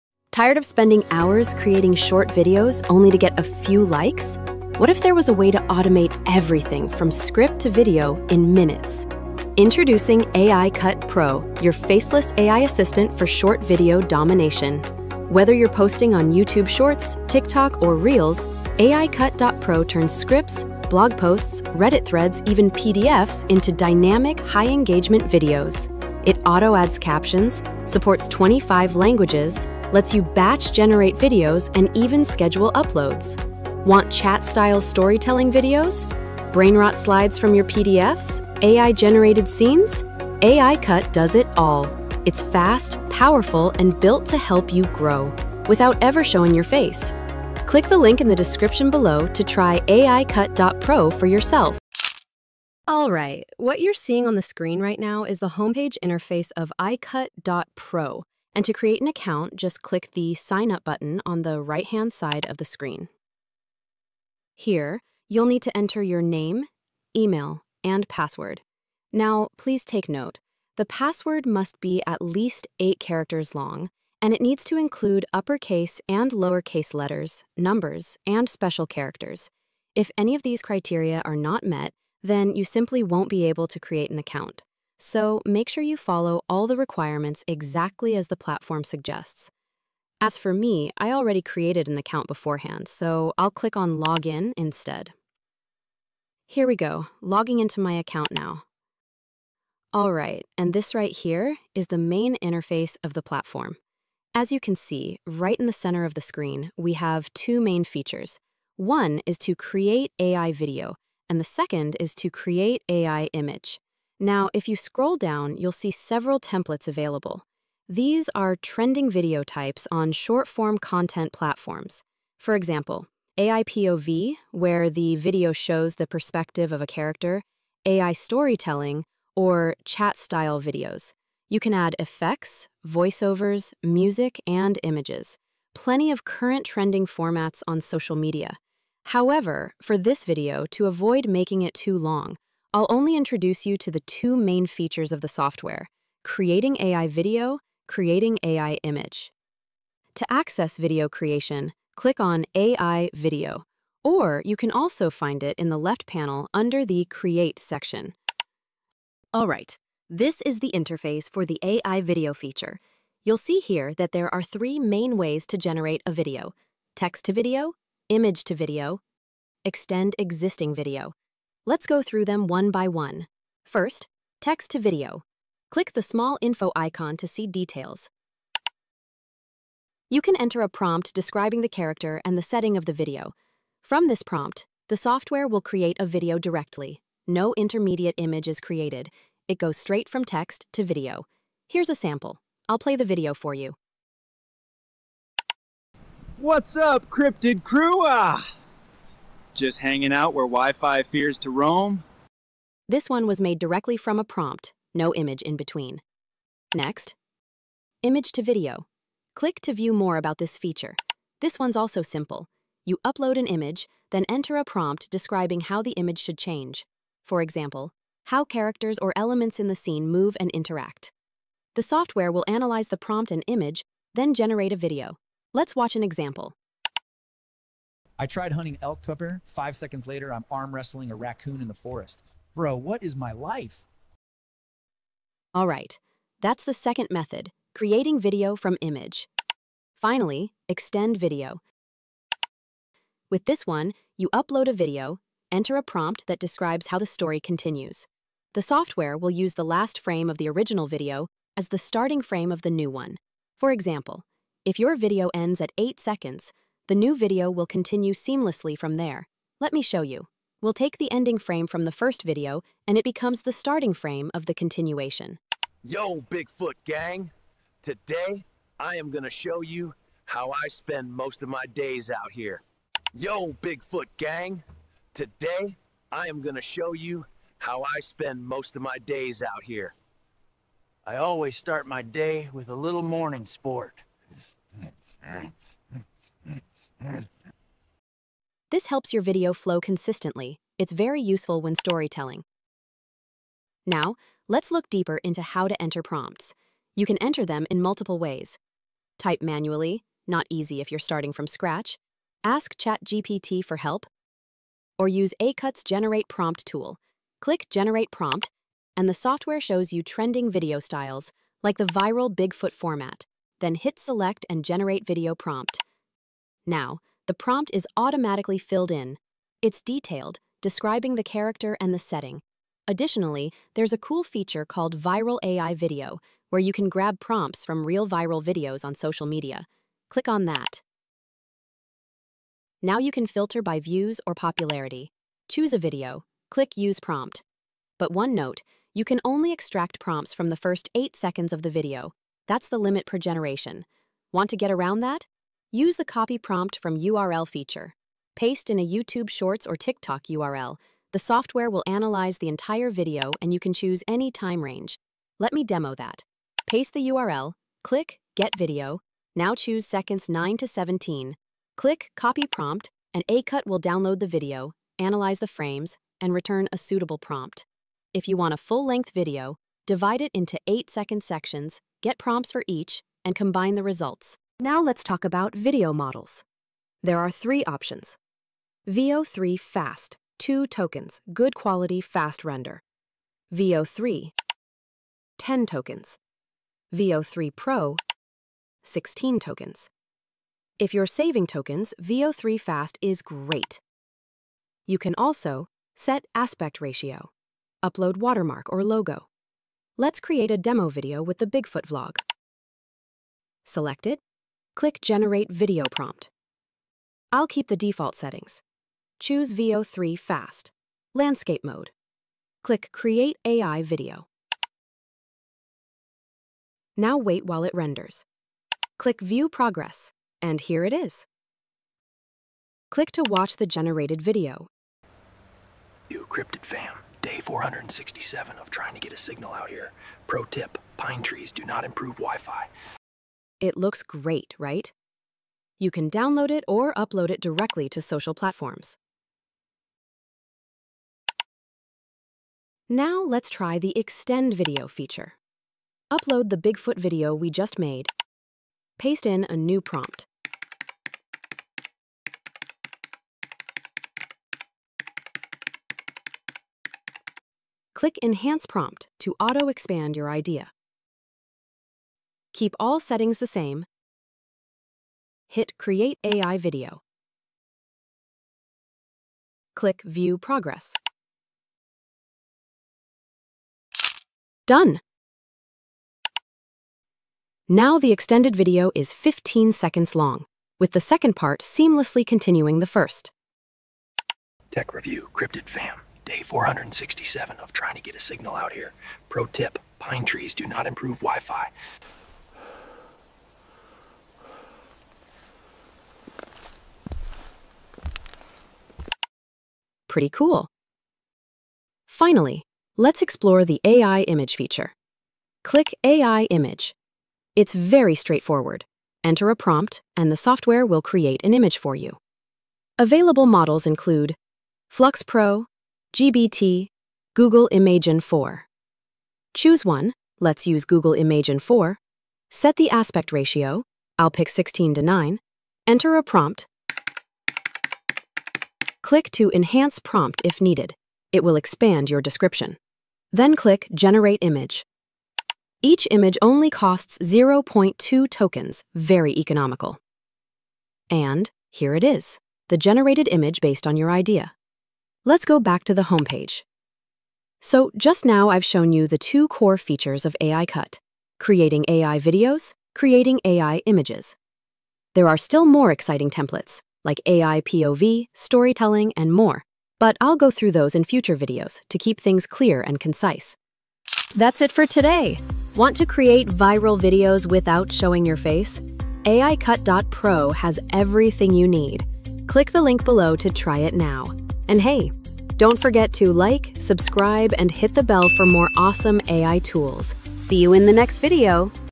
Aicut Review - This Text to Video AI Tool is a Game Changer
Looking for a powerful text to video AI that can create stunning visuals from just a simple prompt? In this video, I’ll walk you through how to use Aicut, one of the most intuitive text to video AI platforms available today.

From generating video from text, transforming images into dynamic clips, to extending scenes — Aicut does it all. Whether you're creating content for TikTok, YouTube Shorts, or Instagram Reels, this text to video AI can save you hours of editing time.

I’ll also show how to leverage Aicut’s viral prompt library and URL analyzer to create videos that actually go viral. If you're serious about content creation and want a reliable text to video AI, you’ll love what Aicut can do!

0:52 - Sign up
1:41 - Main features overview
2:54 - Text-to-Video feature
3:27 - Image-to-Video feature
4:00 - Extend Video feature
4:54 - How to generate prompts
5:22 - Clone viral video
5:43 - Copy prompt from URL
6:39 - Demo: Creating a video
7:22 - Demo: Extending a video
8:21 - AI Image generator feature